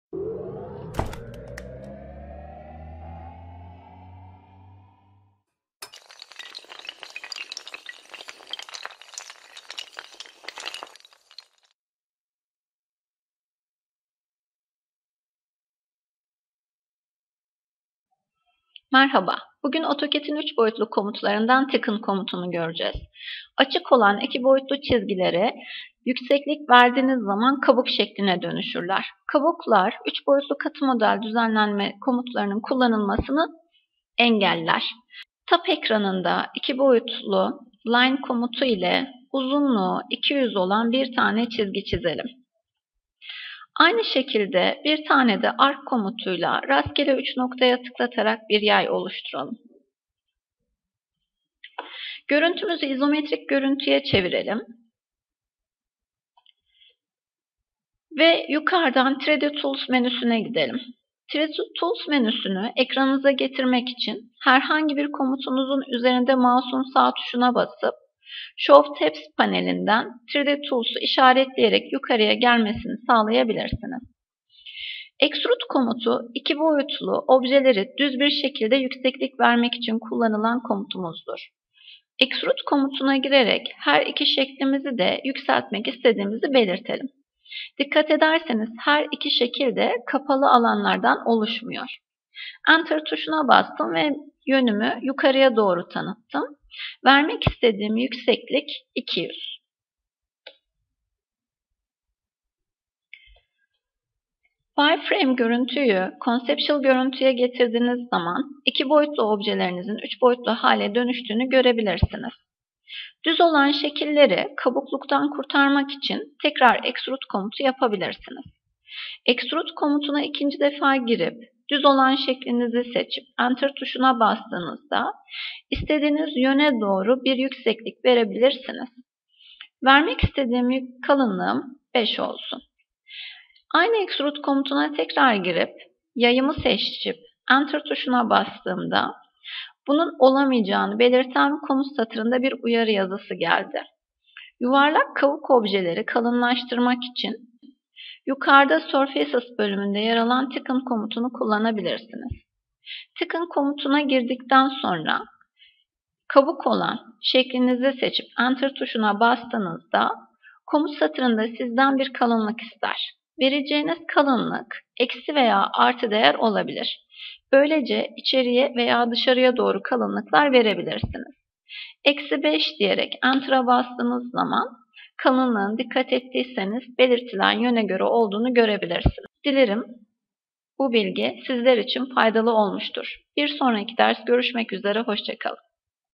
Autocad 3d Thicken Komutu - with English subtitles
Açık alanlardan oluşan iki boyutlu çizgiler, 3 boyutlu hale dönüştürüldüğünde kabuk özelliğinde oluşurlar. Kabuk nesnelerde, katı model düzenleme komutları çalışmaz. Kabukları katı modele dönüştürmek için surfaces bölümünde yer alan thicken komutu kullanılır.